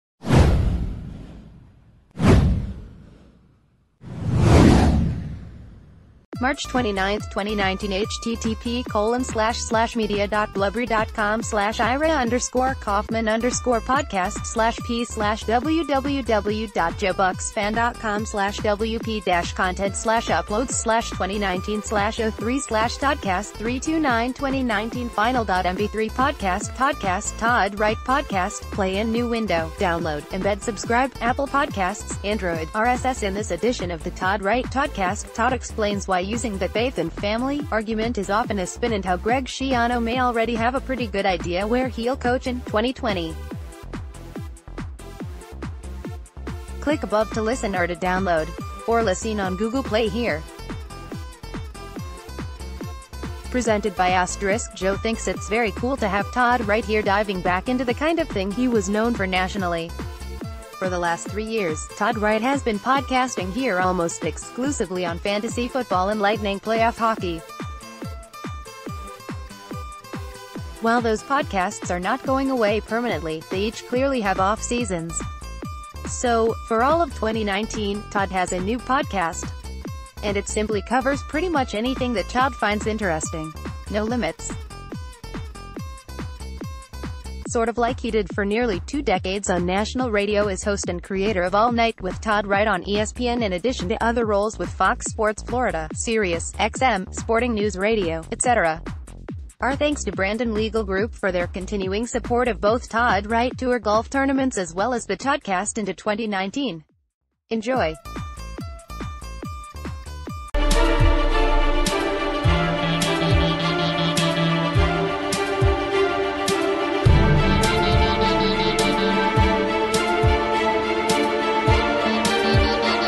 Calling BS On Schiano's Public Reasons For Leaving Patriots
March 29th, 2019 (toddcast-todd-wright-podcast): Play in new window | Download | EmbedSubscribe: Apple Podcasts | Android | RSS In this edition of The Todd Wright TODDcast, Todd explains why using the “Faith and Family” argument is often a spin and how Greg Schiano may already have a pretty good idea where he’ll coach in 2020. Click above to listen or to download. Or lis...